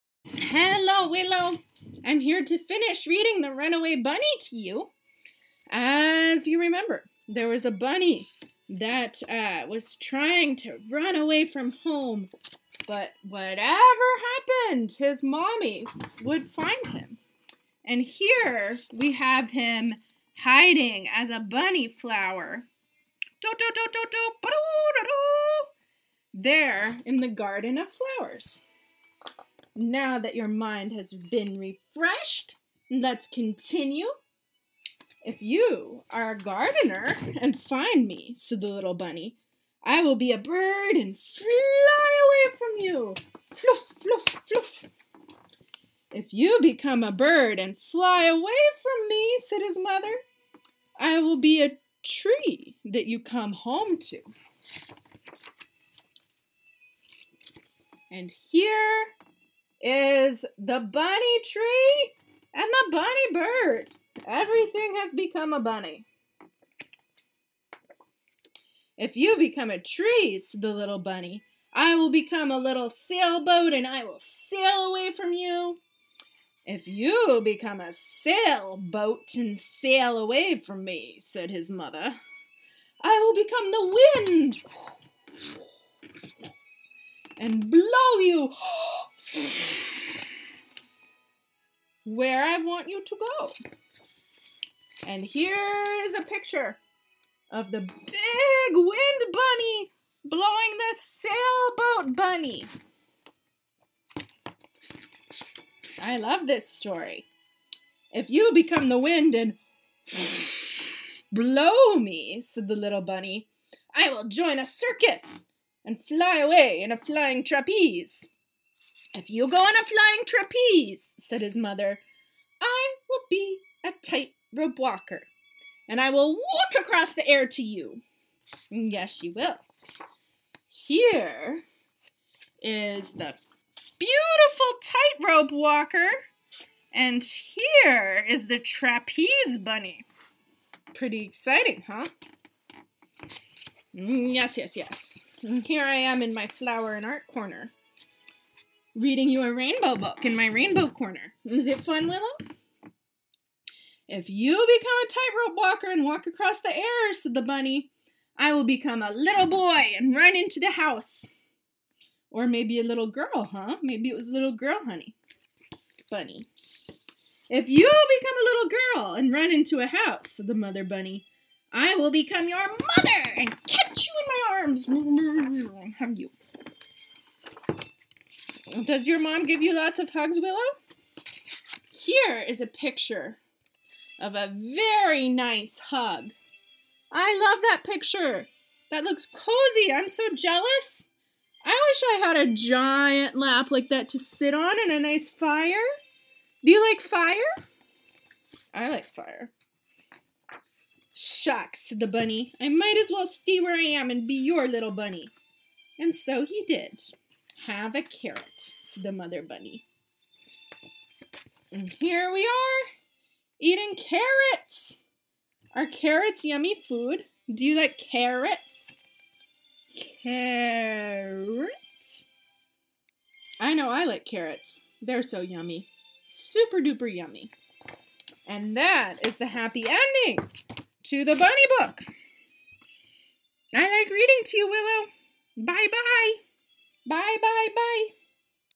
Reading The Runaway Bunny to Willow
I am the fairy god-aunt of an amazing baby named Willow. I tried to read the first half of this story to her over Marco Polo but this turned out to be the wrong medium as Willow became intensely frustrated attempting to grab the phone out of mom's hands. This is episode two of The Runaway Bunny for your desktop viewing which we hope will be a less frustrating medium for the little ones. Note we have also increased production values with a hair and makeup department and a subtle soundtrack.